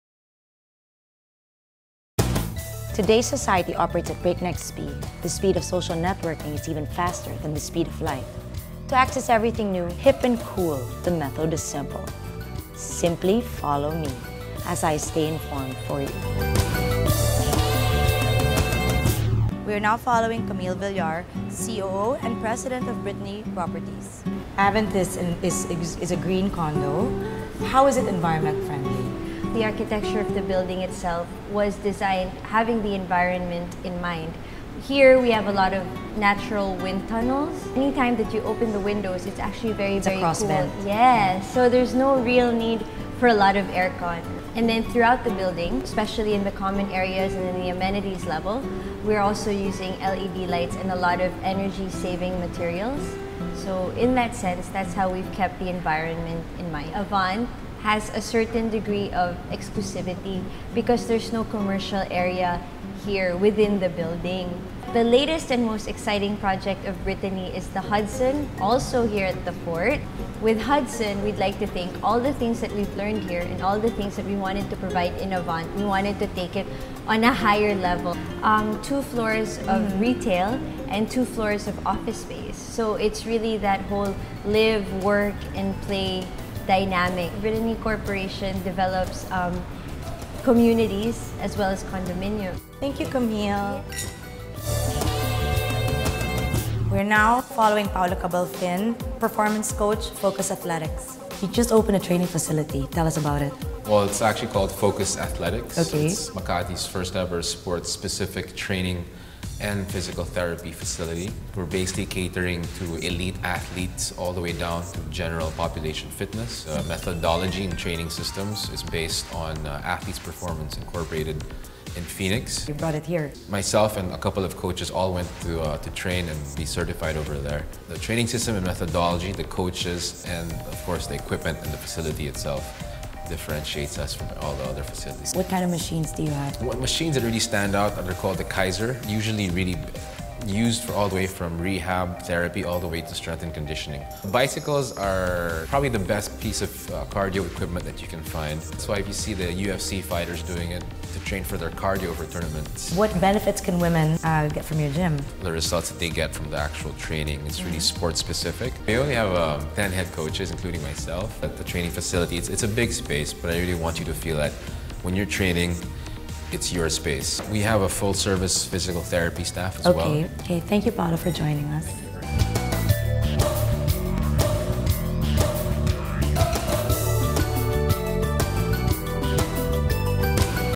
Follow EP 7 - September 5, 2012
The 7th Episode of Follow with guests

1. Camille Villar
2. Paolo Cabalfin of Focus Athletics